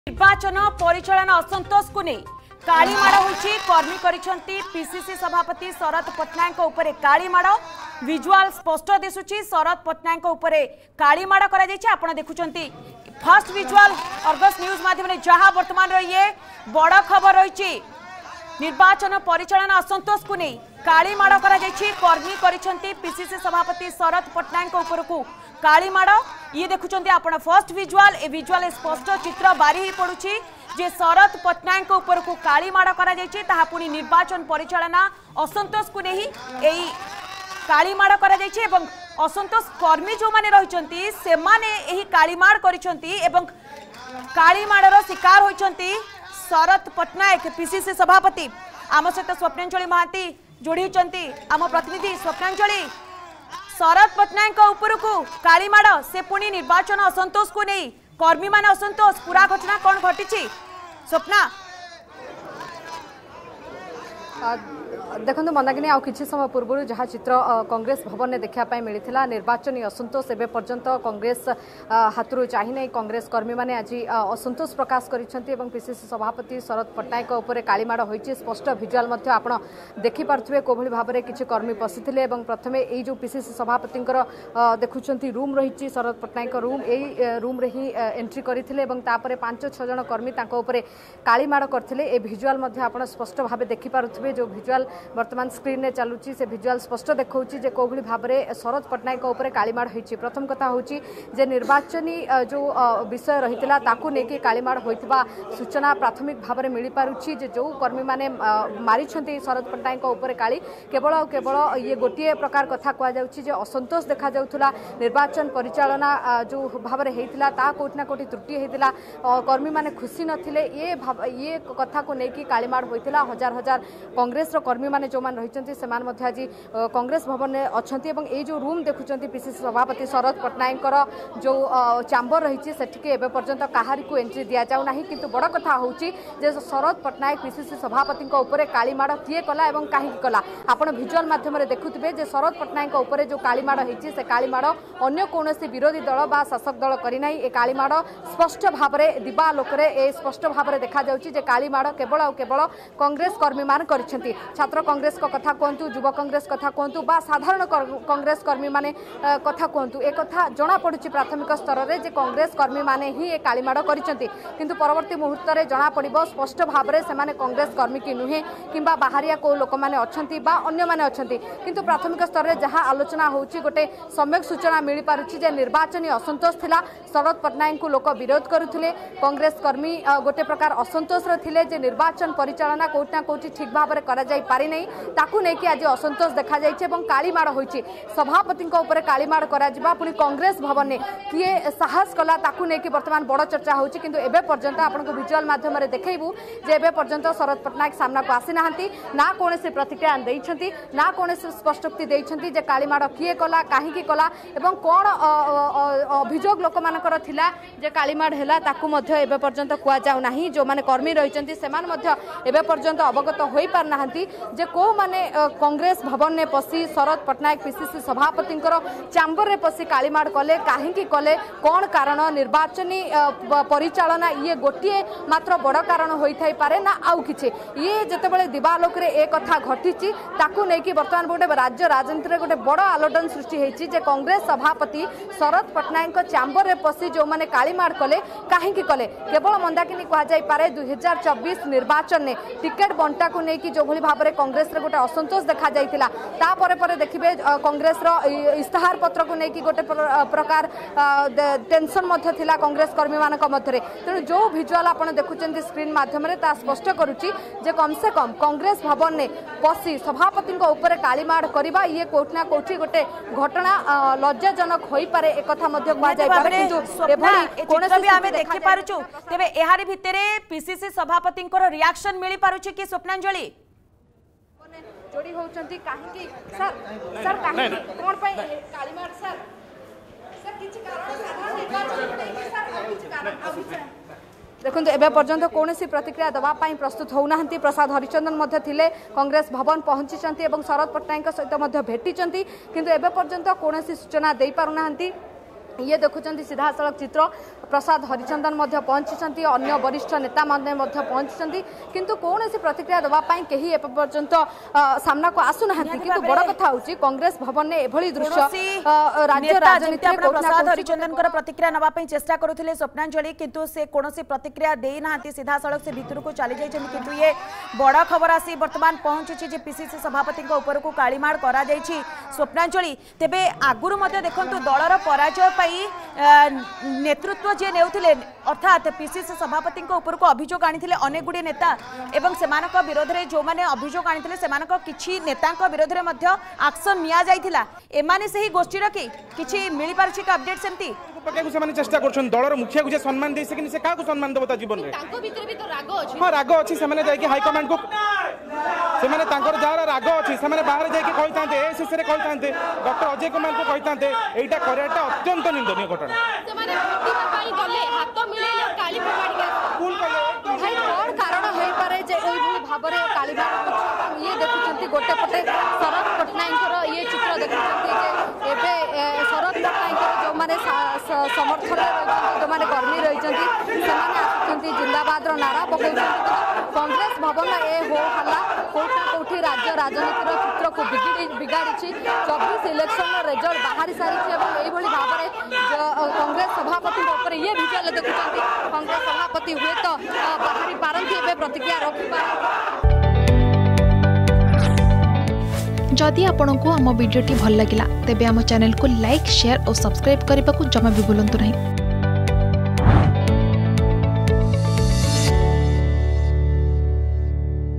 ପିସିସି ସଭାପତିଙ୍କୁ କାଳିମାଡ || Congress || Sarat Pattanayak
ନିର୍ବାଚନ ପରିଚାଳନା ଅସନ୍ତୋଷକୁ ନେଇ ପିସିସି ସଭାପତି ଶରତ ପଟ୍ଟନାୟକଙ୍କ ଉପରେ କାଳି ମାଡ଼ ହୋଇଛି।